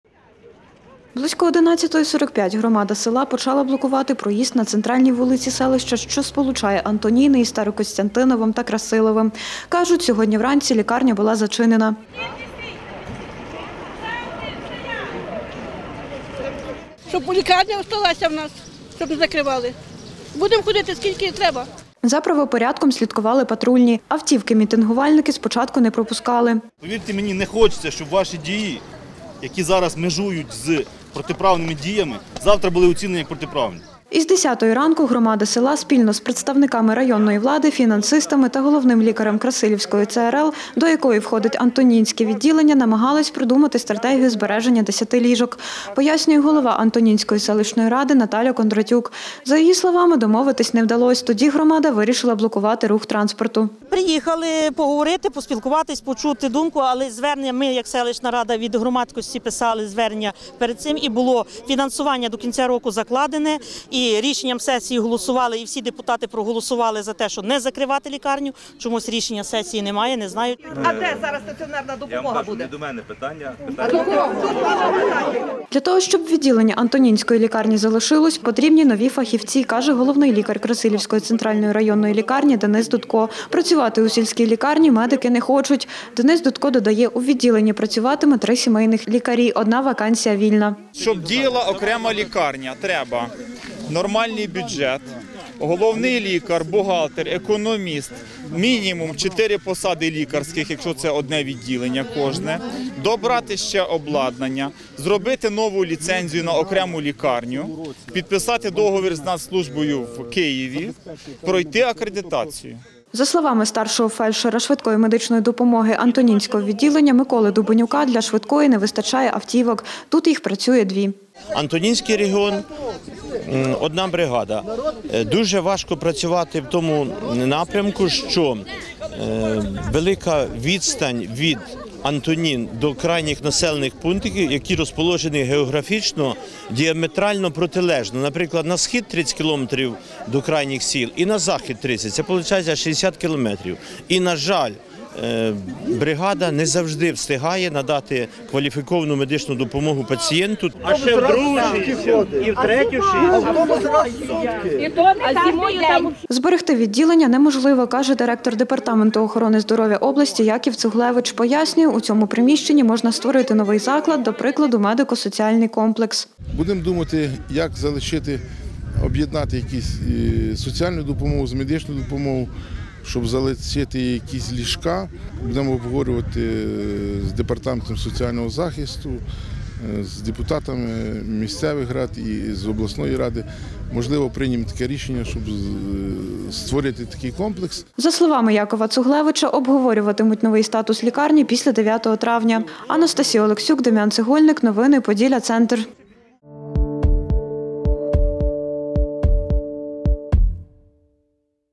Жителі села Антоніни сьогодні блокували рух автомобільного транспорту
Протягом двох годин жителі села Антоніни сьогодні блокували рух автомобільного транспорту на центральній дорозі селища. Головна вимога – збереження 10-ти стаціонарних ліжок Антонінського відділення Красилівської центральної районної лікарні, каже голова селищної ради - Наталя Кондратюк.